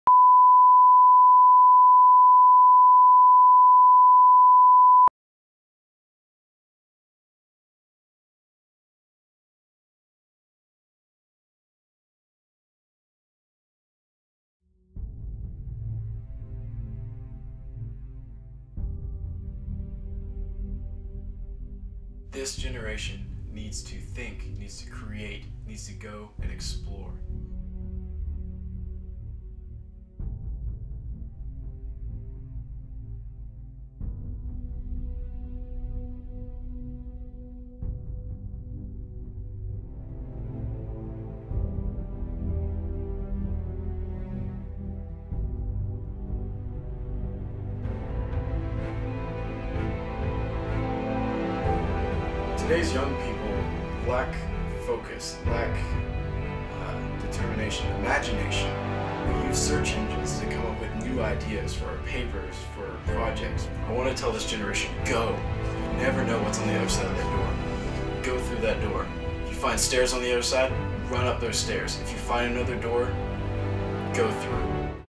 THINK. CREATE. EXPLORE.
Please ignore the BARS and tones, they are a requirement for the beginning of my projects. May need volume up. A PSA for COMS 333. I wanted to emphasize a break from mere distractions and "fillers" of our time so we can use our imaginations and explore new things.